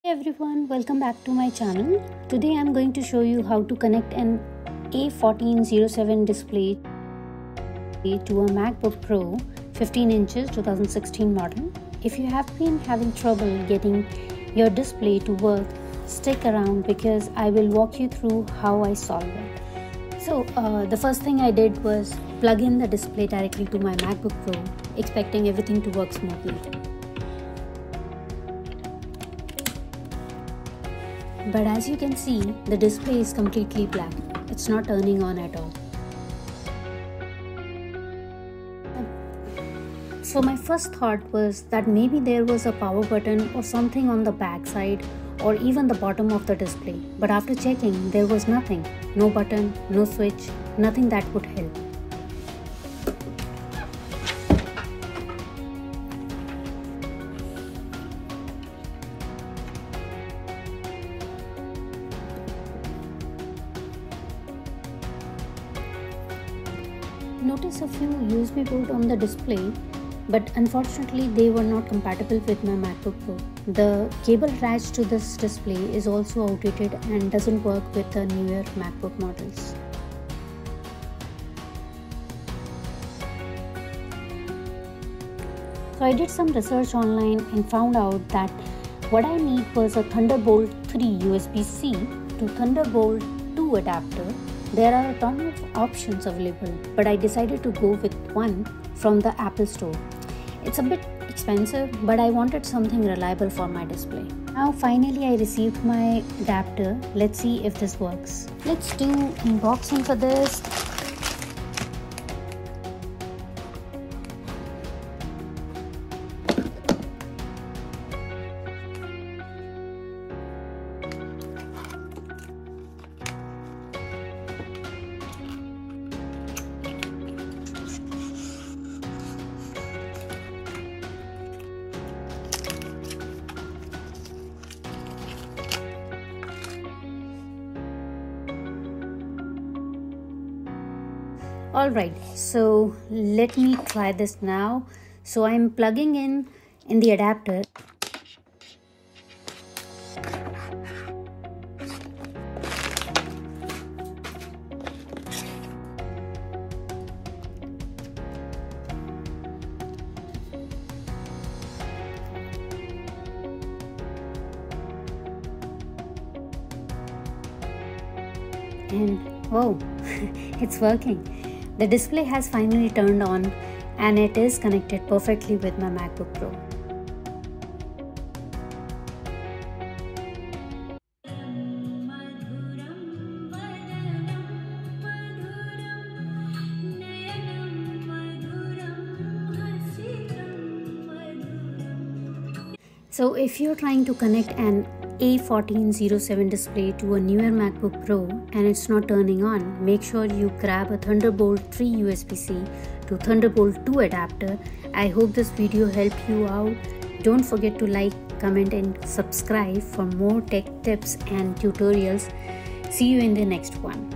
How to Connect an A1407 Display to a MacBook Pro (15-inch, 2016)
In this video, I'll show you how to connect an A1407 display to a MacBook Pro (15-inch, 2016). When I first tried to connect the display, it didn't turn on, and I couldn't figure out what was wrong. After checking for any buttons or settings on the display itself, I realized that the outdated cable and ports were not compatible with my MacBook. I then researched online and found out that I needed a Thunderbolt 3 (USB-C) to Thunderbolt 2 adapter to make it work. Watch as I walk you through the entire process, from troubleshooting to getting the display up and running. If you're facing the same issue, this video will guide you through the steps to successfully connect your A1407 display to your MacBook Pro.

Thunderbolt 3 (USB-C) to Thunderbolt 2 Adapter